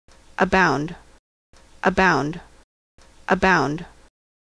How to Say or Pronounce Abound
This video shows you how to say or pronounce Abound.

How would you say Abound?